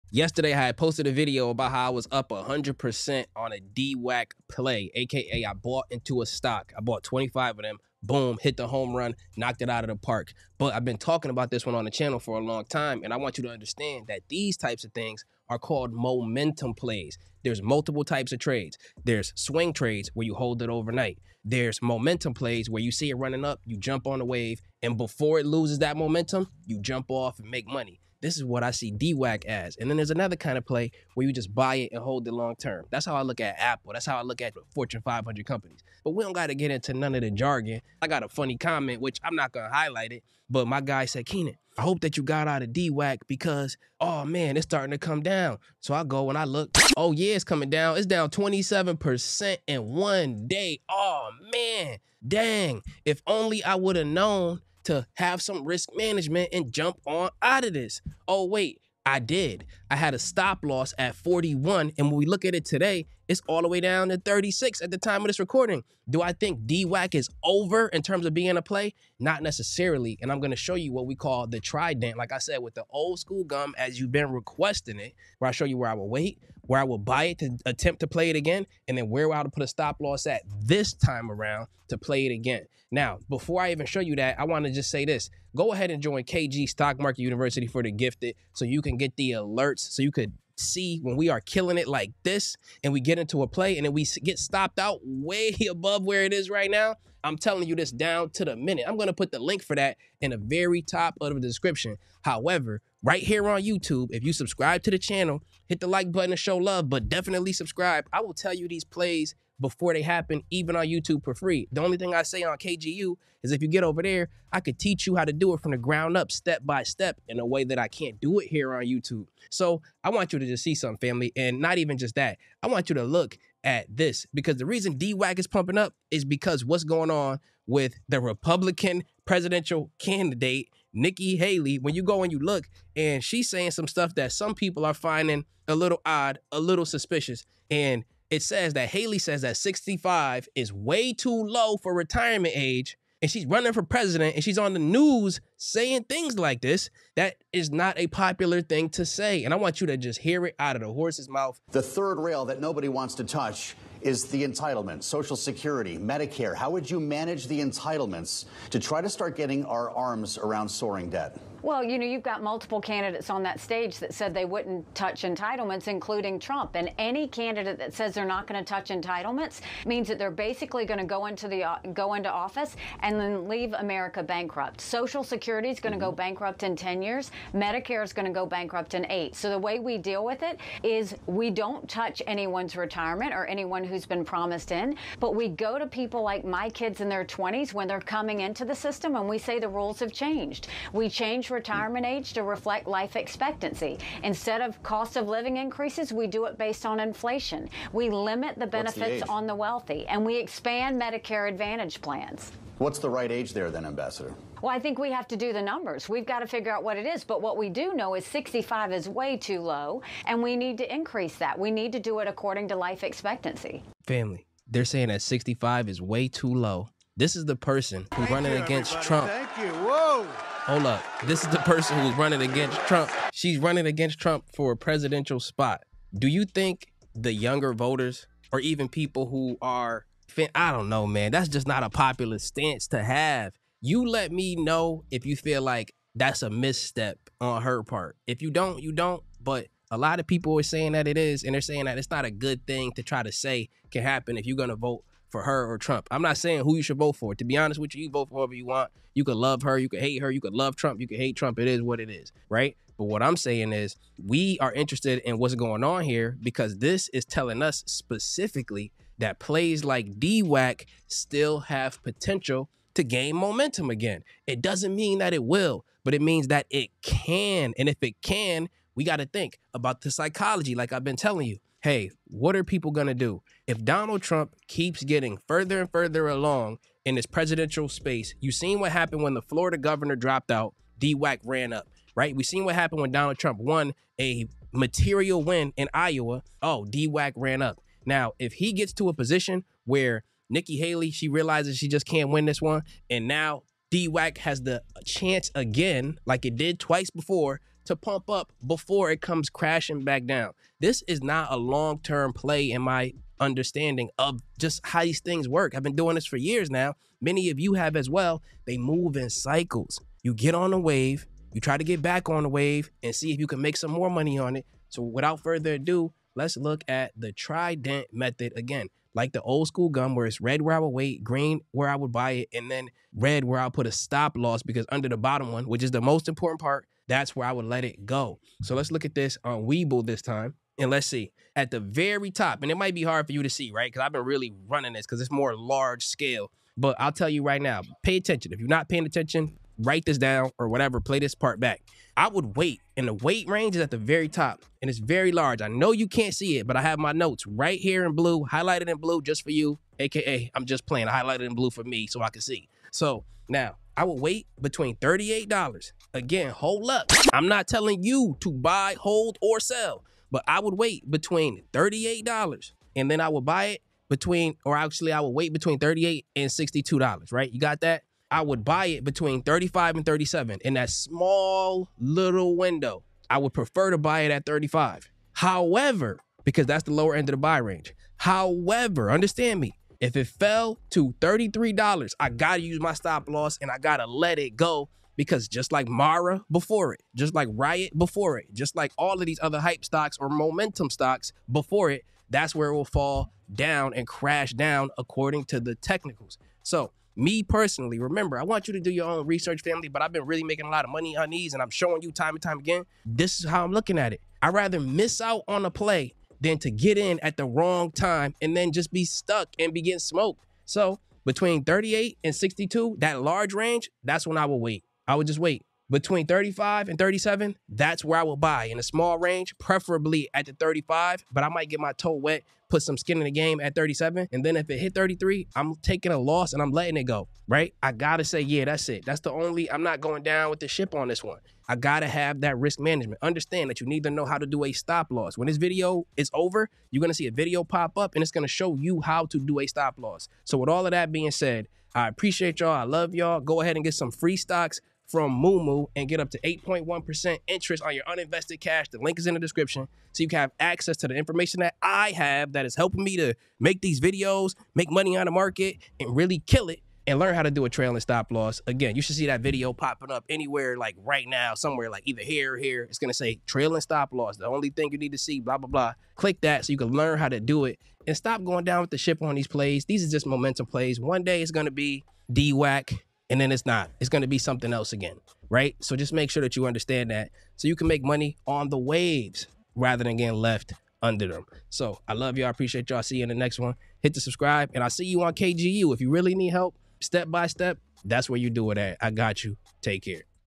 URGENT DWAC UPDATE | BUY NOW Or Wait? | +Stop Loss!
Embark on a comprehensive exploration of DWAC stock with Kenan Grace in this enlightening YouTube video. Kenan delves into the intriguing dynamics surrounding presidential candidate Nikki Haley and former President Trump, shedding light on how political news influences the stock market. Gain valuable insights into trading strategies, effective risk management, and stay updated on the latest stock market news. Discover the nuanced interplay between political developments and market trends in this insightful discussion.  @clearvaluetax9382 DO NOT FILE YOUR TAX RETURN YET